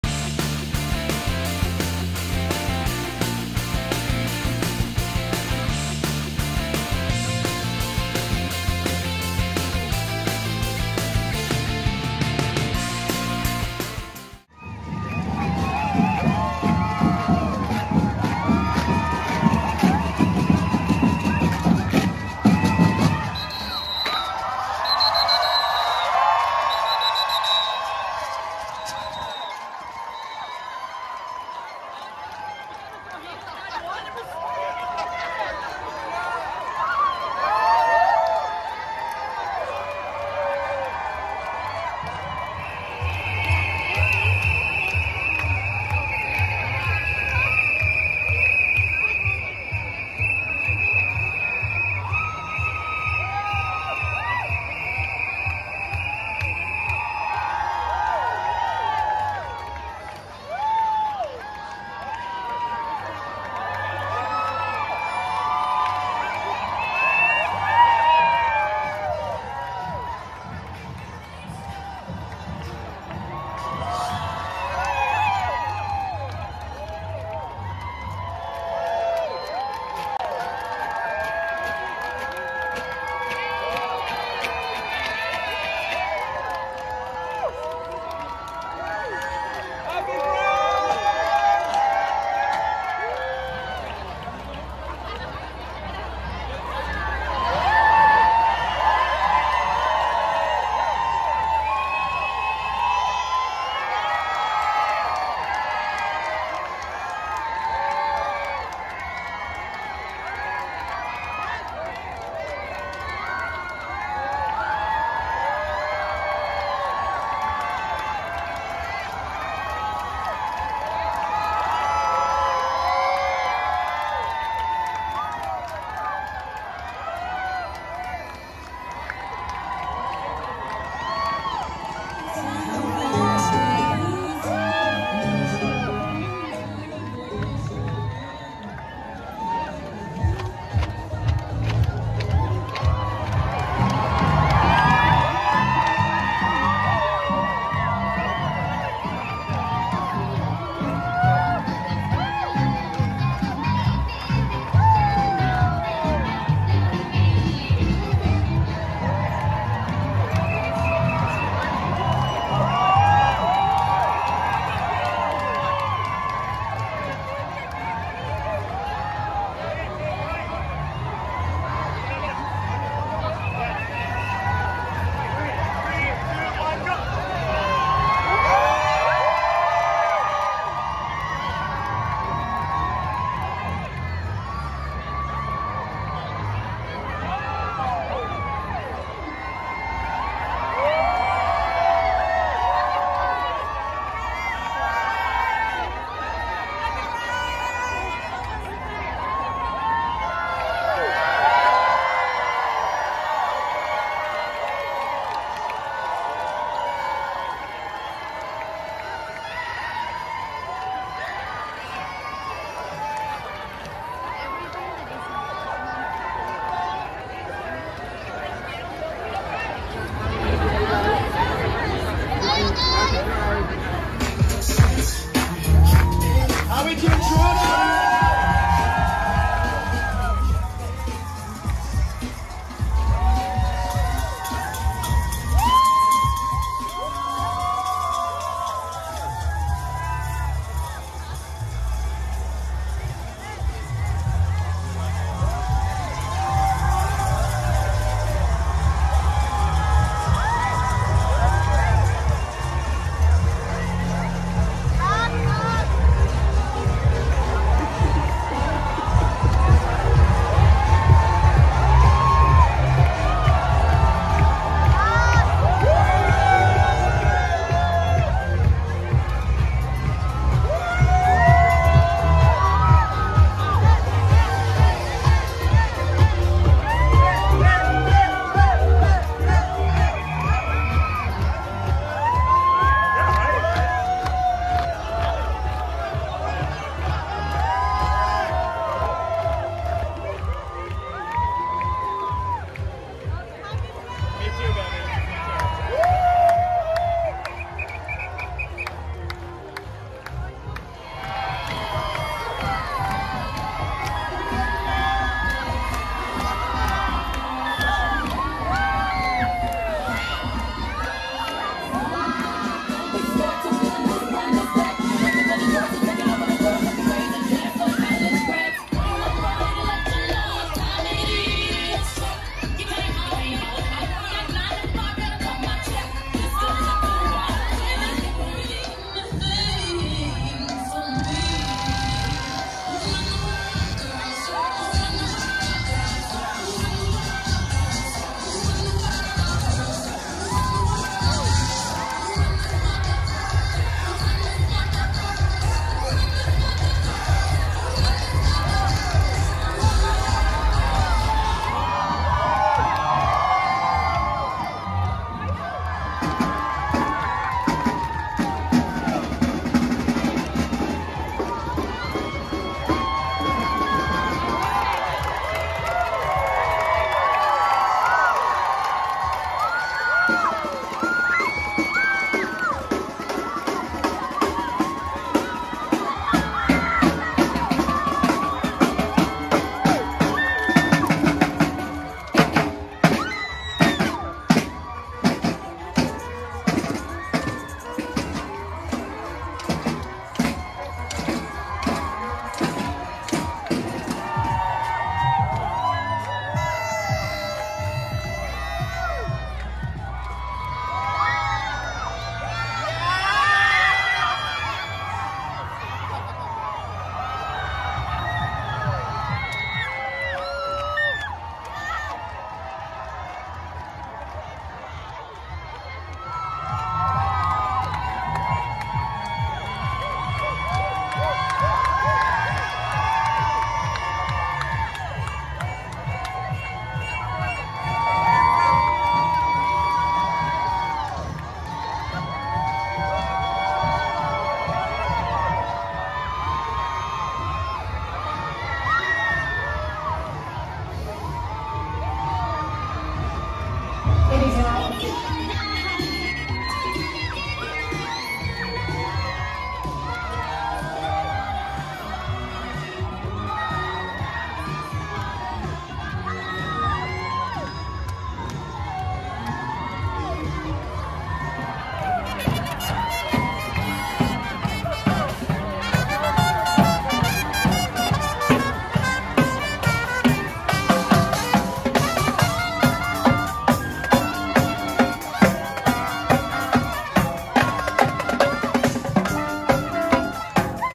Pride Festival Toronto Canada
Watch the street festival of Pride 2022 Toronto. Take a walk with me!